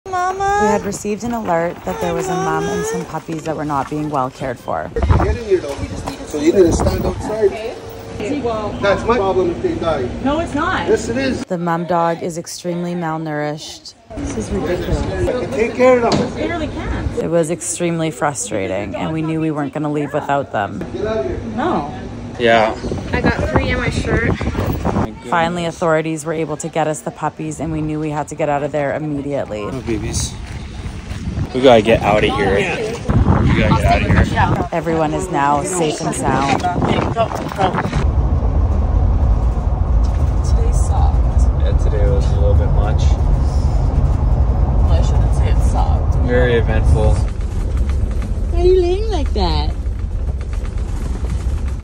Couple Refuses To Leave Without Rescuing Dog Mom And Puppies | The Dodo
They’re now on their way to their forever home ❤️ @brady.oliveira